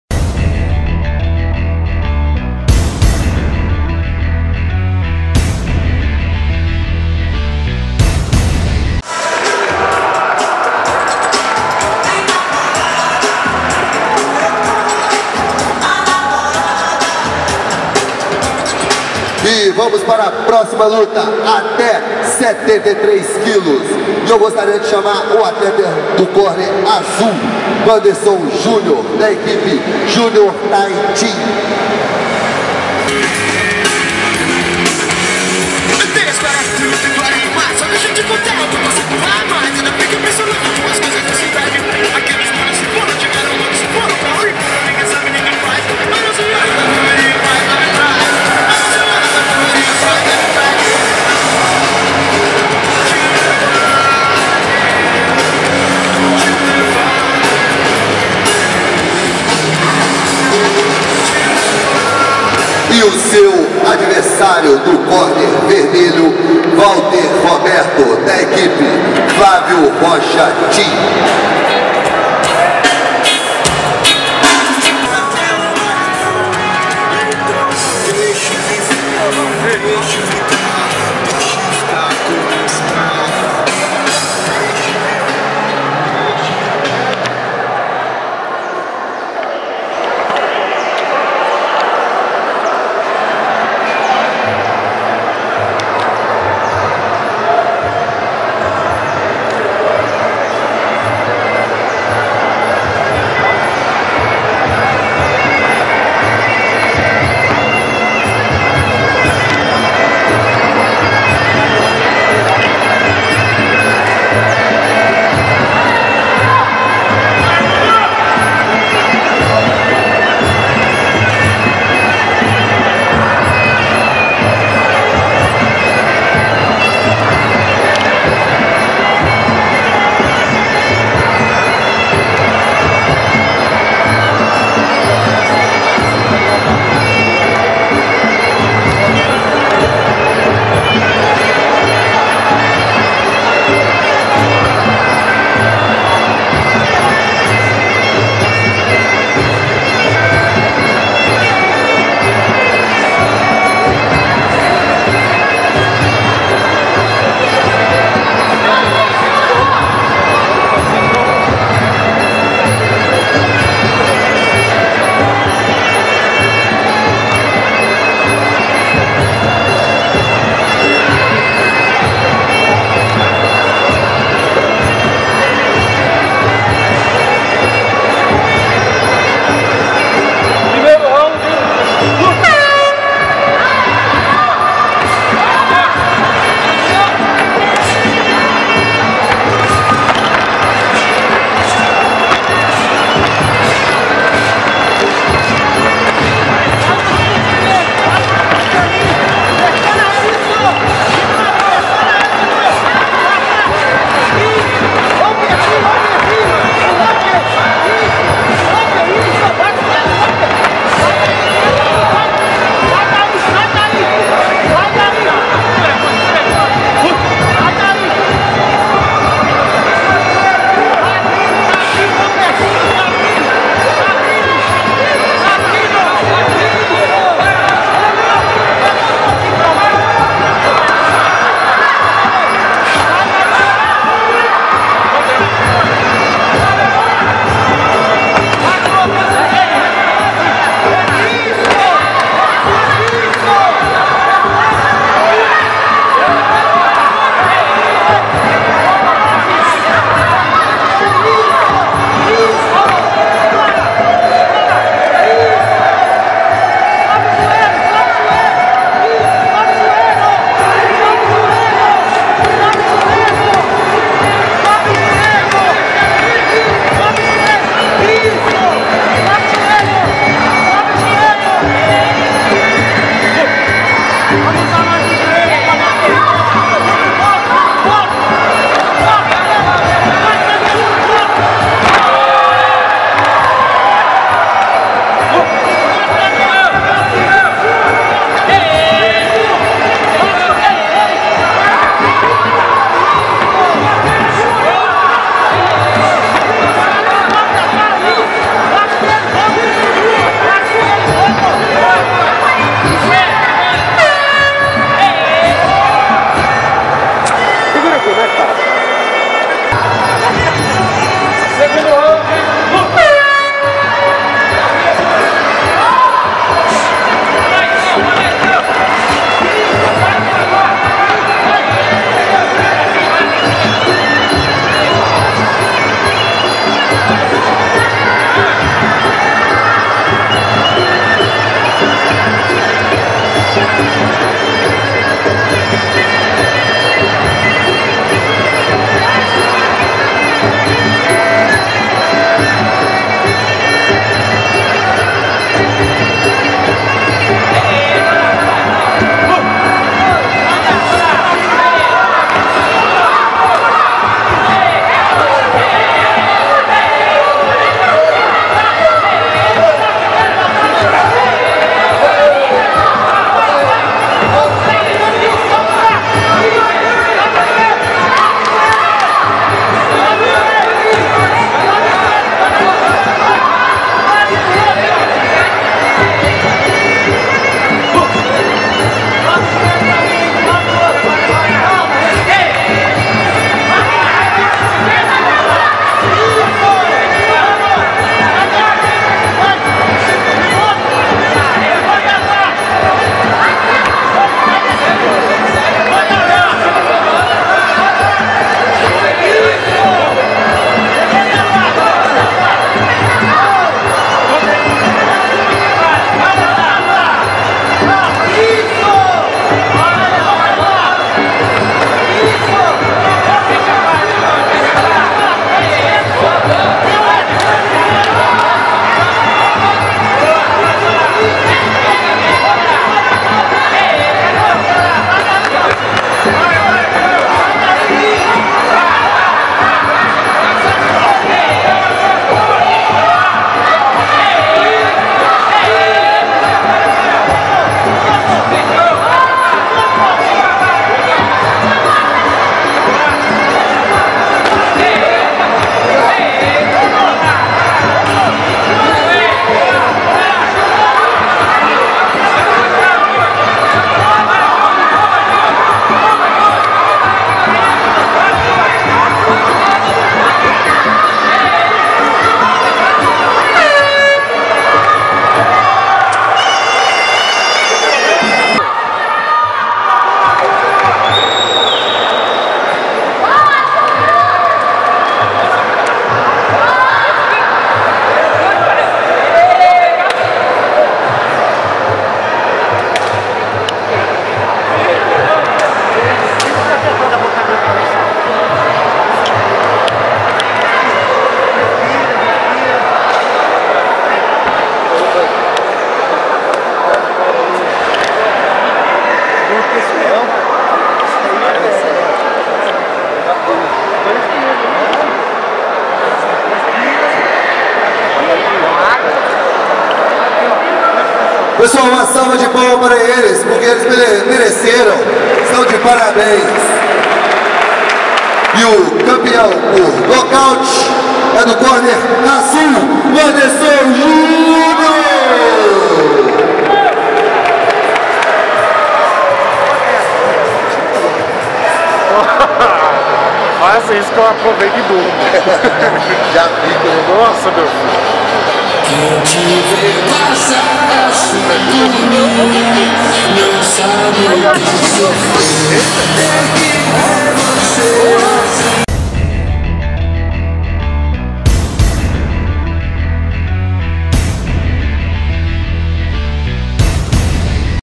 Wanderson vs Valter - Império MuayThai 5
Em combate de MuayThai amador até 73,5kg, Wanderson Júnior (Junior Thai Team) venceu Walter Roberto (Flávio Rocha Team) por nocaute técnico.